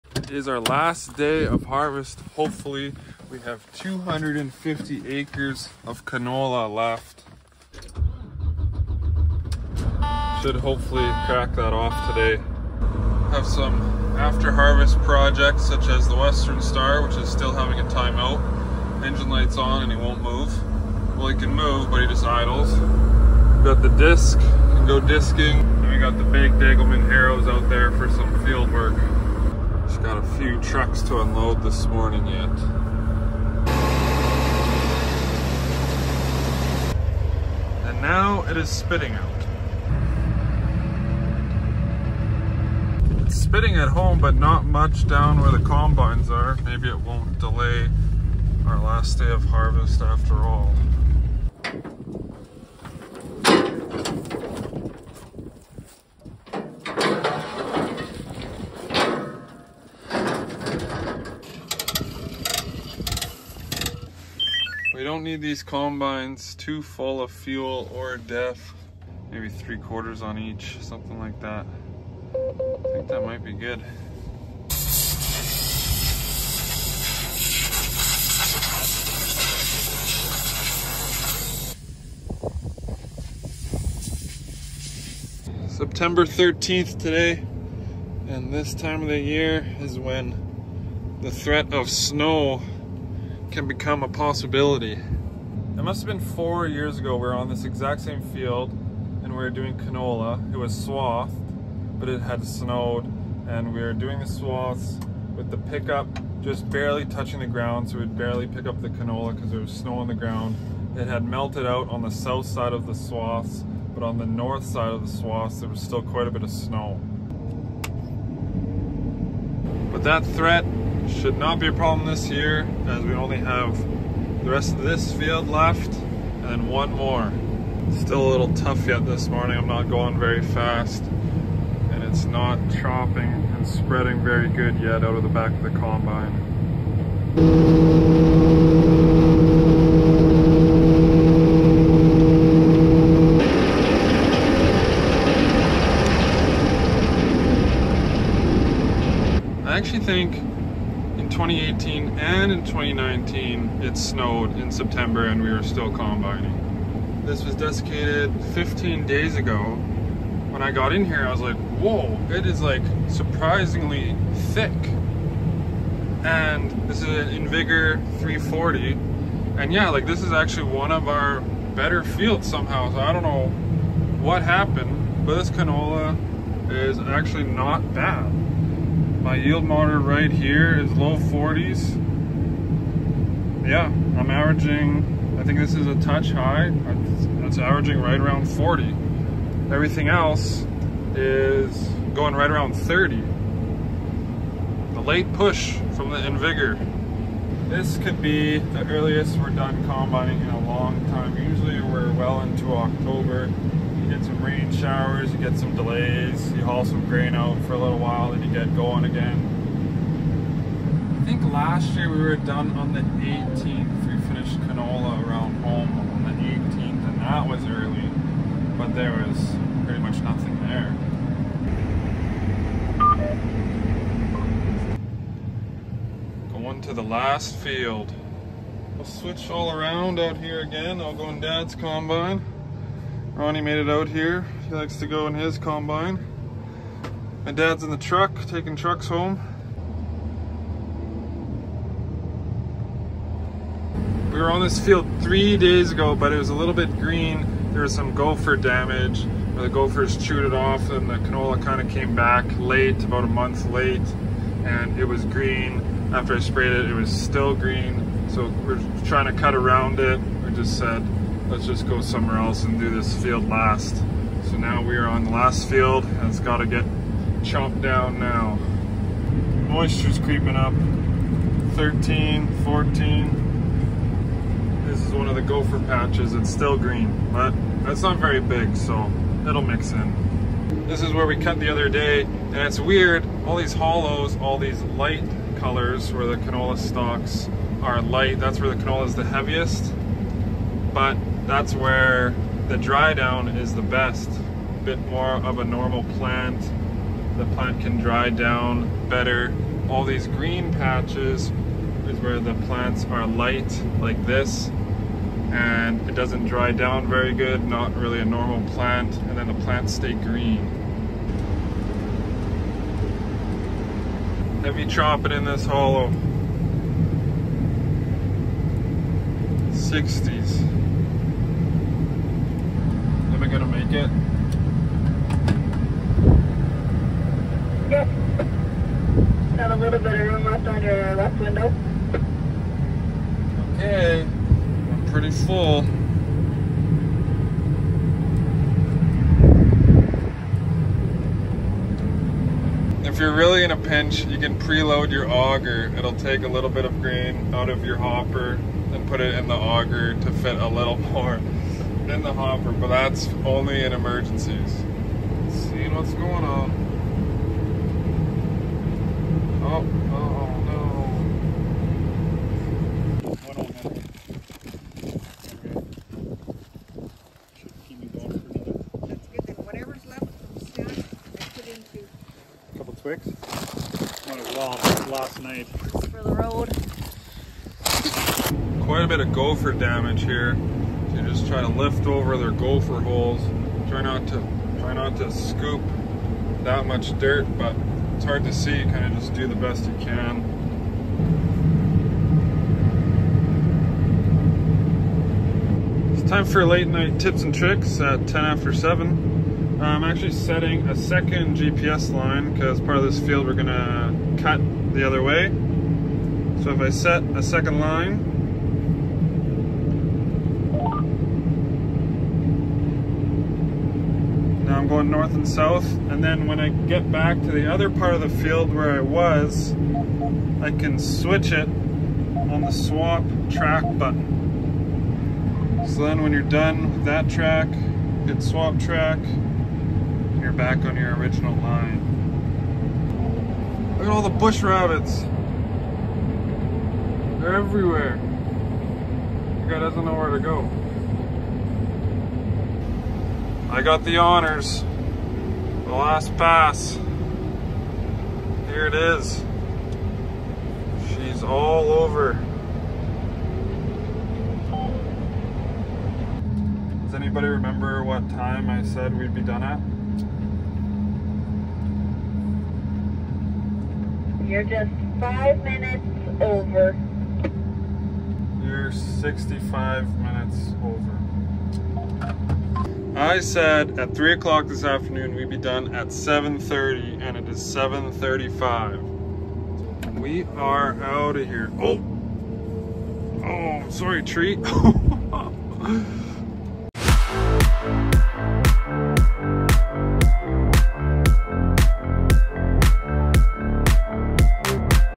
Crossing The Harvest Finish Line
The last day of harvesting canola